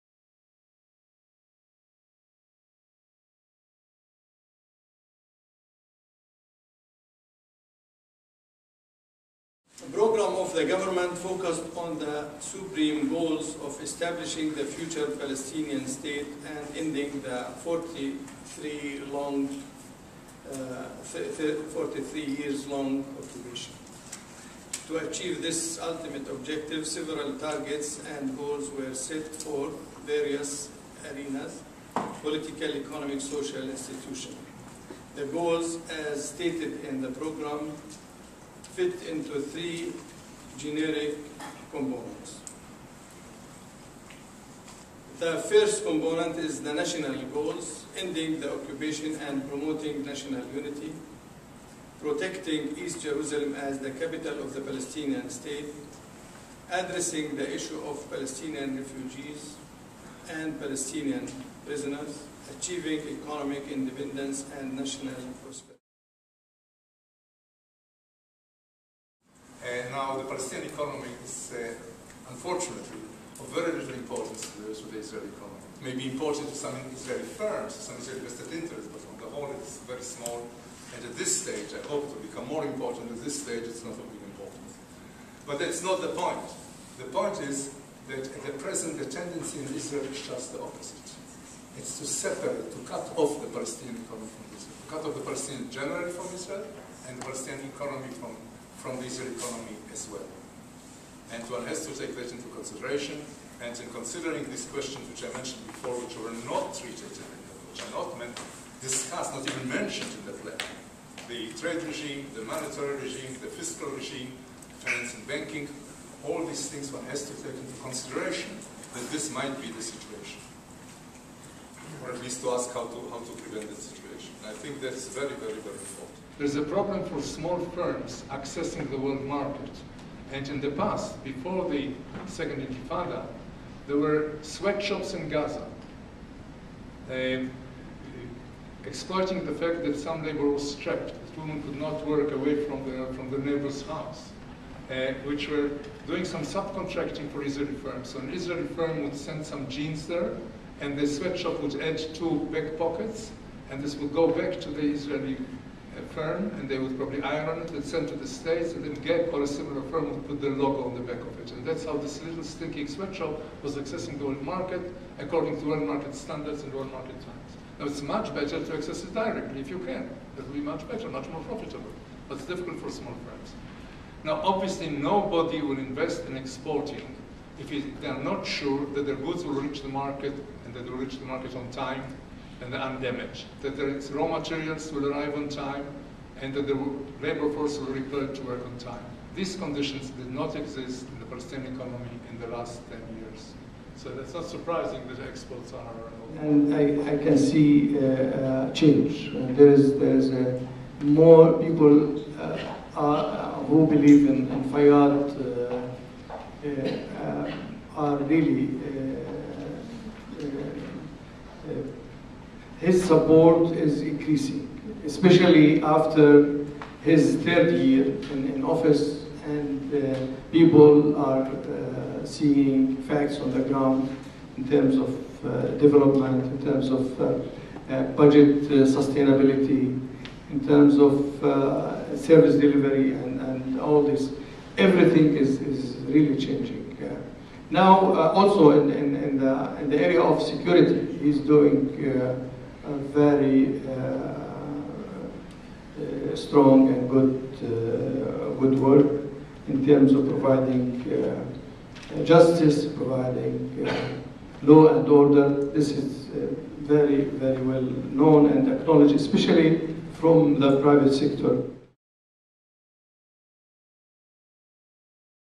Afternoons with IPCRI: Salam Fayyad State-Building Plan (Highlights)
For its Afternoons with IPCRI event on August 18th, 2010, IPCRI hosted Samir Abdullah, former minister of planning for the Palestinian Authority, and Ephraim Kleiman, professor of economics at Hebrew University. They reviewed the progress of Palestinian Prime Minister Salam Fayyad's state-building plan one year after it began. Highlights of their remarks are available here.

The Afternoons with IPCRI series is made possible by the governments of Finland and the Netherlands.